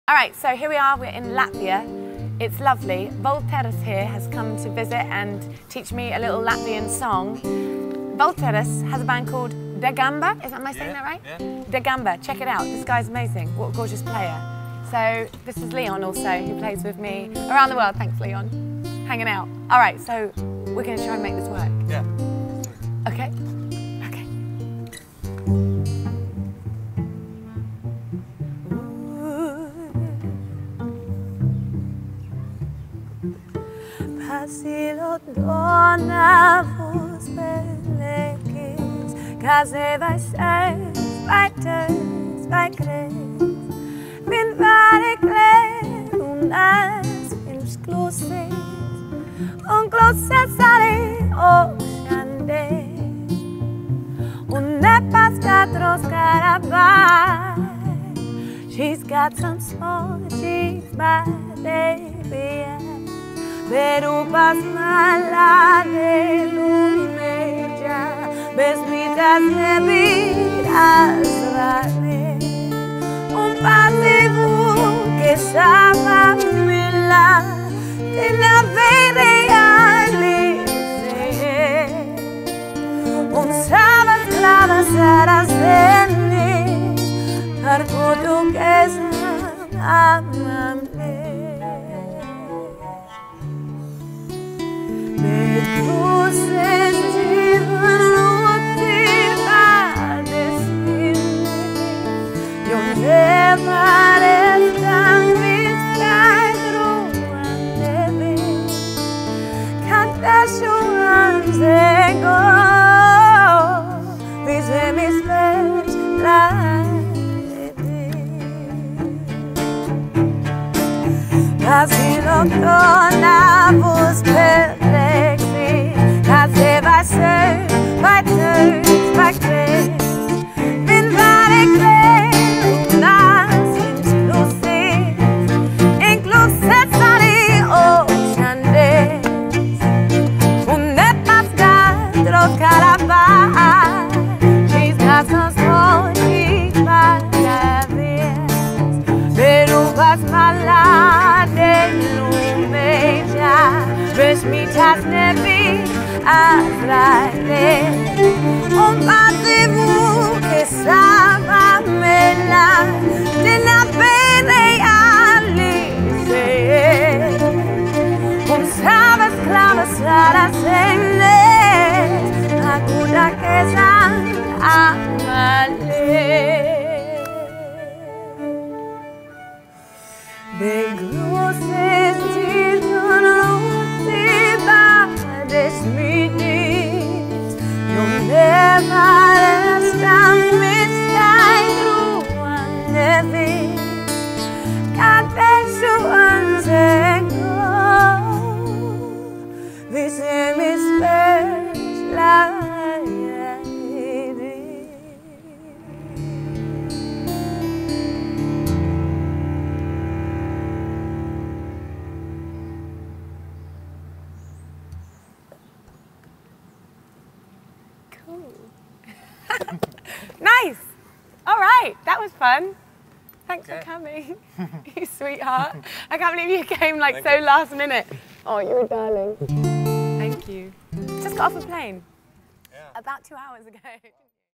Valters Puce ft. Joss Stone - Latvia
Latvia, here we did a collaboration with Valters Puce, he is part of the group, DAGAMA. They fuse musical cultures to form a unique and powerful sound. His passion and motivation for music is seen in this video. Have a listen and sure to be inspired by this type of music, we hope you enjoy!